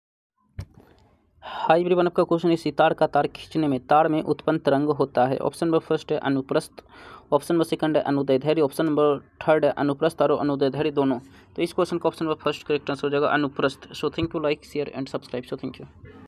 सितार का तार खींचने में तार में उत्पन्न तरंग है ?
• अनुप्रस्थ

• अनुदैर्ध्य

• अनुप्रस्थ और अनुदैर्ध्य

• इनमें कोई नहीं